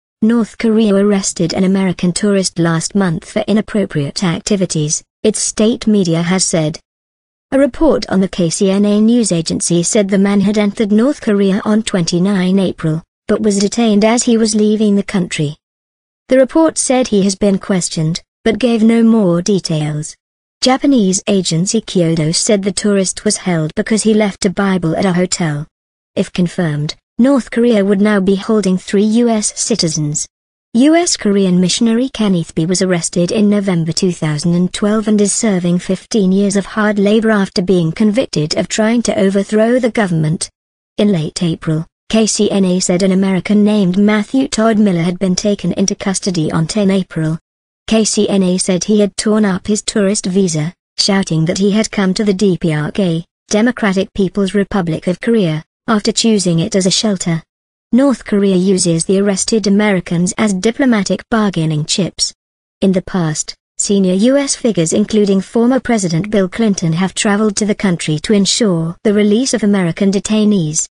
North Korea 'detains third American
North Korea arrested an American tourist last month for inappropriate activities, its state media has said.

A report on the KCNA news agency said the man had entered North Korea on 29 April, but was detained as he was leaving the country.

The report said he has been questioned, but gave no more details. Japanese agency Kyodo said the tourist was held because he left a Bible at a hotel.

If confirmed, North Korea would now be holding three US citizens.

US-Korean missionary Kenneth Bae was arrested in November 2012 and is serving 15 years of hard labour after being convicted of trying to overthrow the government.

In late April, KCNA said an American named Matthew Todd Miller had been taken into custody on 10 April.

KCNA said he had torn up his tourist visa, shouting that he had "come to the DPRK [Democratic People's Republic of Korea] after choosing it as a shelter".

North Korea uses the arrested Americans as diplomatic bargaining chips.

In the past, senior US figures including former president Bill Clinton have travelled to the country to ensure the release of American detainees. .